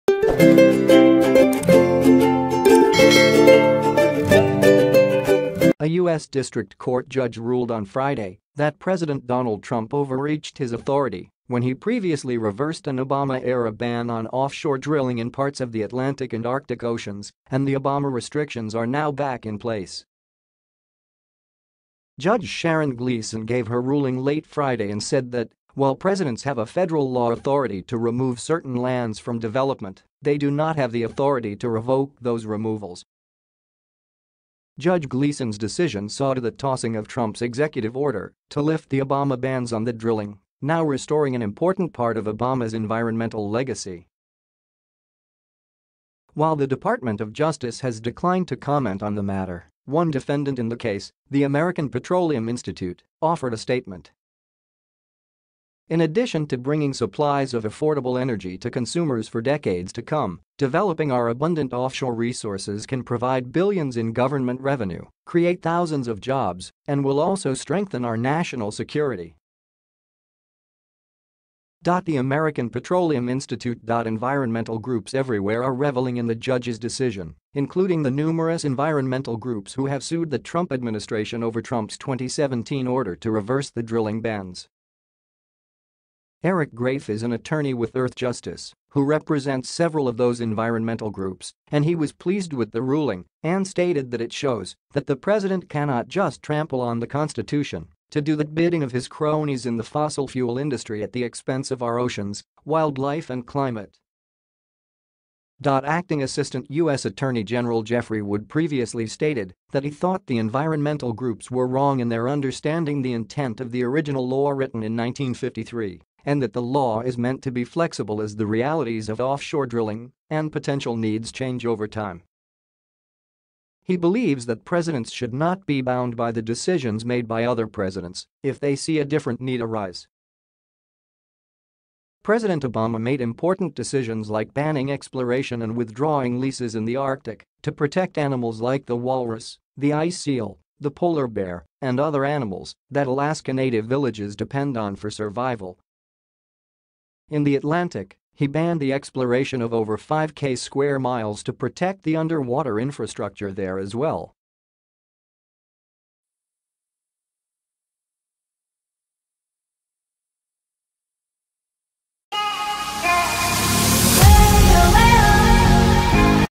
OBAMA JUST SCORED A MAJOR VICTORY OVER DONALD TRUMP
A US District Court judge ruled on Friday that President Donald Trump overreached his authority when he previously reversed an Obama-era ban on offshore drilling in parts of the Atlantic and Arctic oc